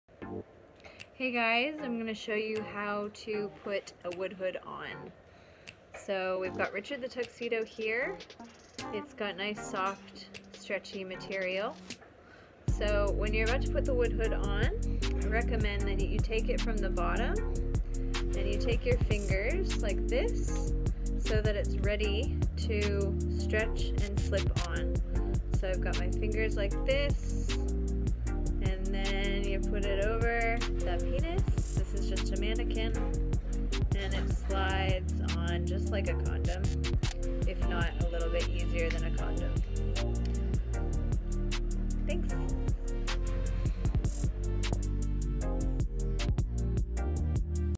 How to put a Penis Costume On
So you got your WoodHood. "How do I put this thing on?" you might ask? Check out how LT, one of the creators of WoodHoods, puts a penis costume on - aka a WoodHood.
Note: The wood knob is just a mannequin hehehe!

Want to see more WoodHoods? Check out the full range on our

Socials:
Facebook.com/Theoutfitforyourwilly